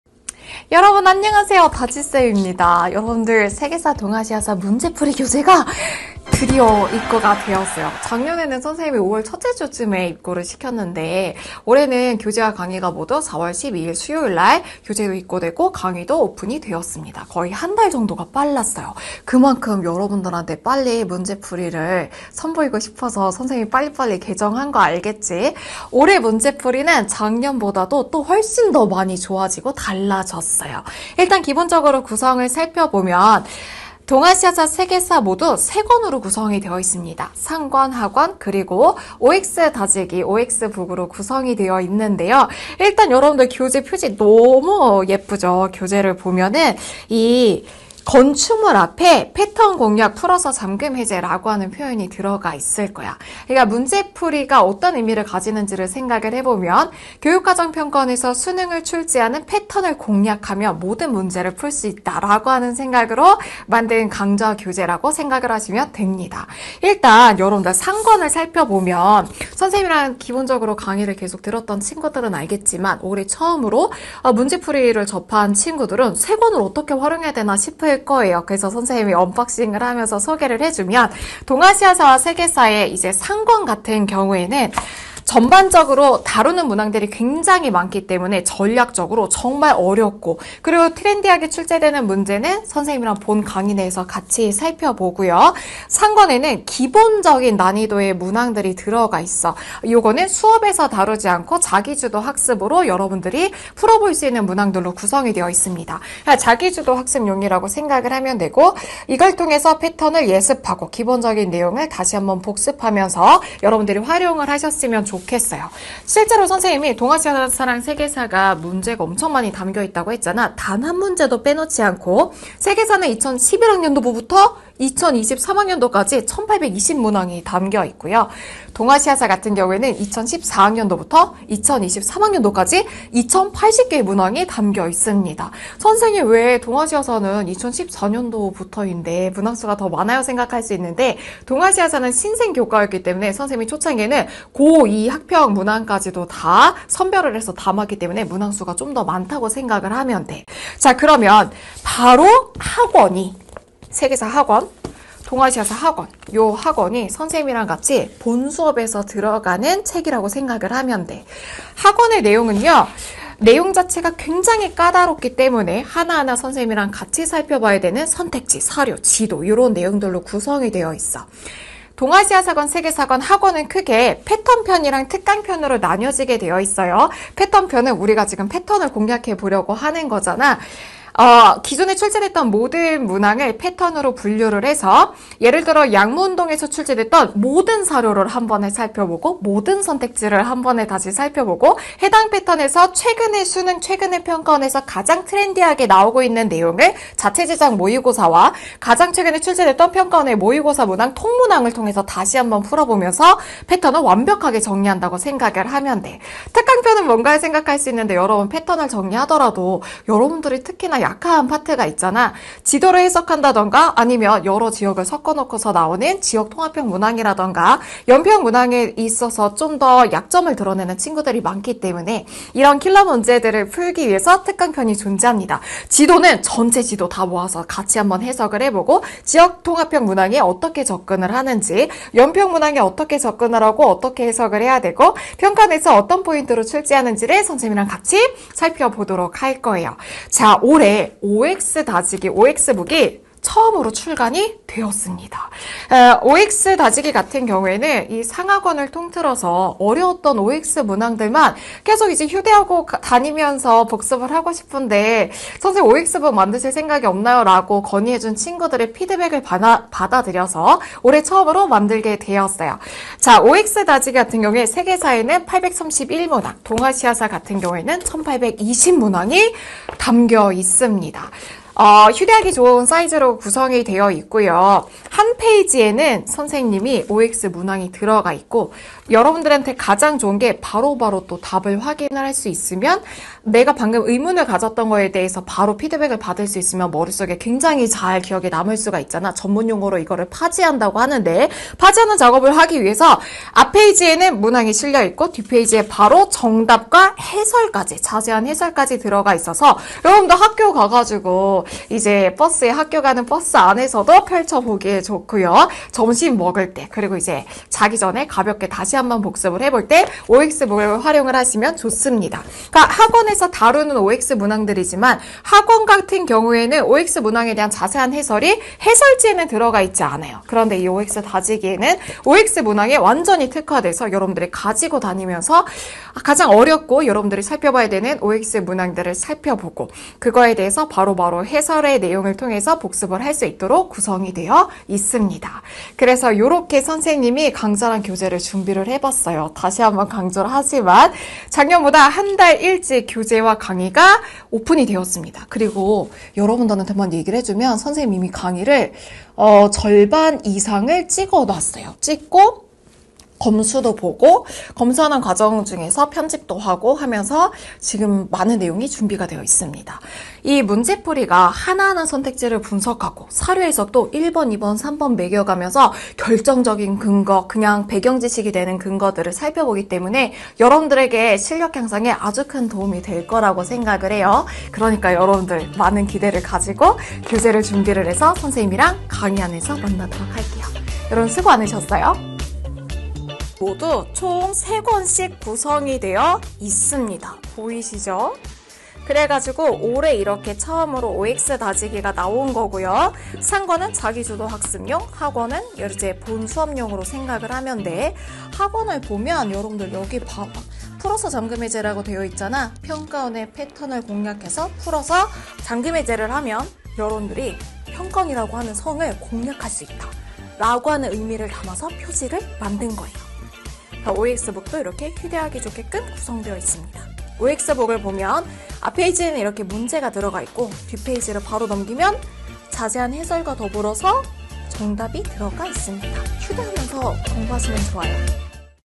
[메가스터디] 역사 이다지 쌤 - 동사&세사 이다지도 탁월한 문풀 교재 언박싱♥
드.디.어

이다지도 탁월한 문제풀이가 개강&교재 입고 되었습니다! (짝짝짝~)

올해 더욱 업그레이드 된 탁월한 문제풀이 교재,

다지쌤이 직접 알려주는 교재 활용법까지!

[교재 특징]

1. 체계적인 학습이 가능한 교재 구성

2. NEW 휴대가 가능한 OX 다지기로 언제 어디서든 학습 가능!

짹짹이들~ 우리 강의에서 만나용♥

- 역사 이다지 선생님을 더 알고싶다면?